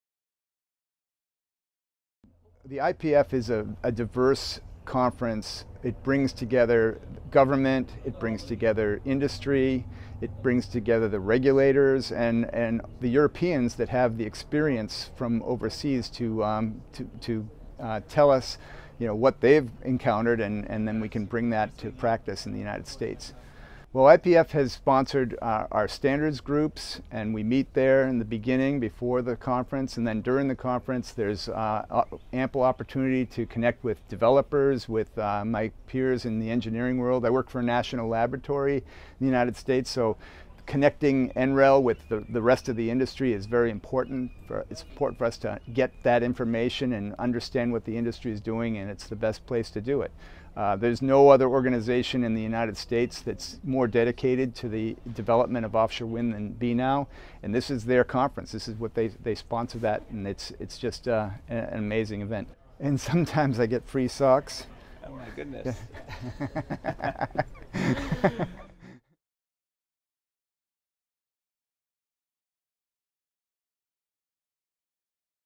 Walt Musial on the perks of the Business Network for Offshore Wind's IPF
Walt Musial (NREL) talks about the perks of attending the IPF